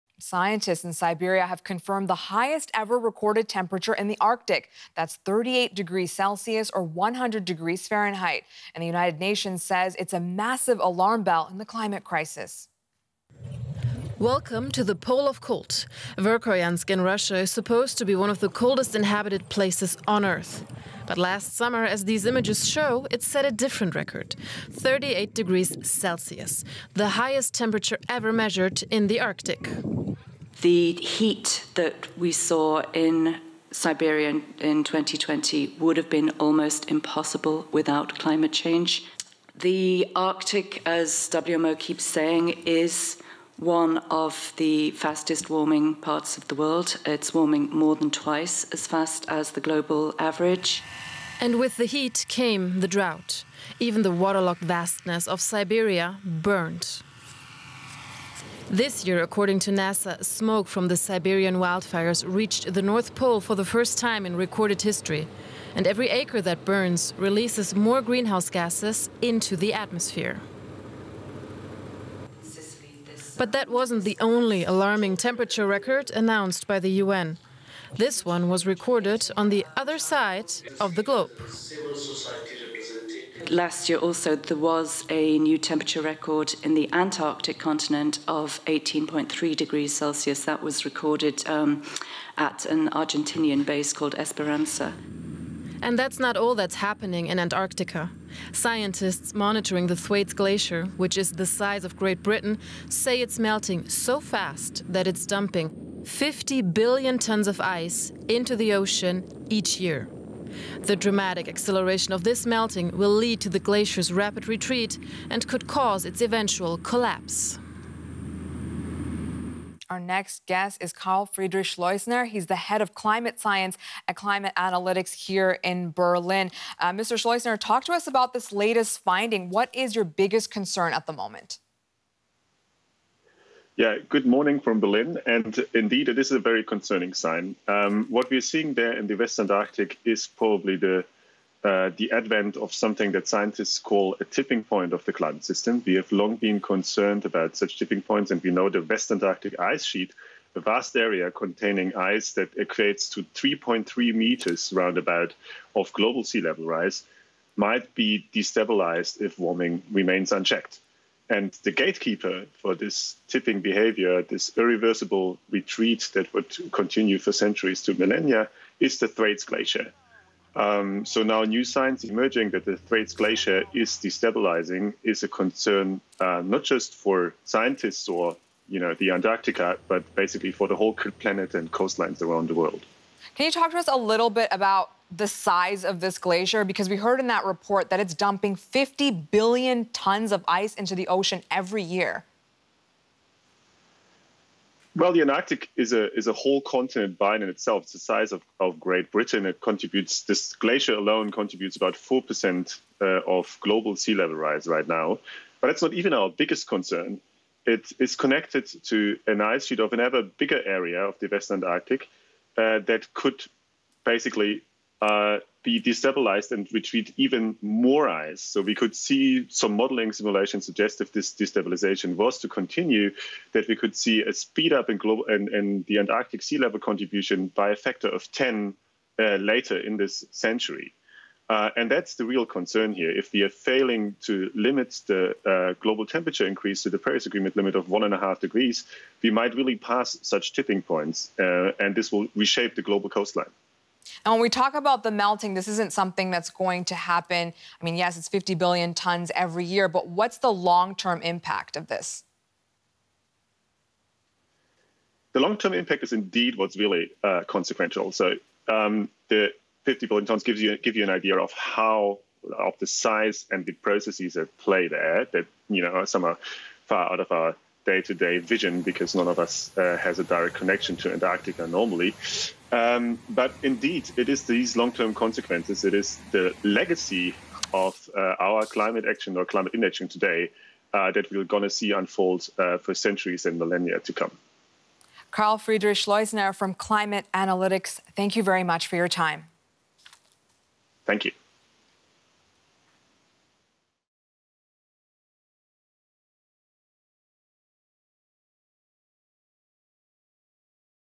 Arctic heat record affirmed: A fuel for climate change? | DW News
The World Meteorological Organization (WMO) has entered a new record into the World Weather & Climate Extremes Archive. In June 2020, the temperature in the Siberian town of Verkhoyansk soared to 38 degrees Celsius (100.4 degrees Fahrenheit)